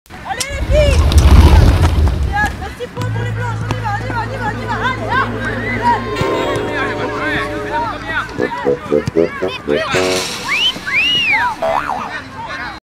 Accélération
Accélération à la Neymar après récupération de la balle ... et but bien sur!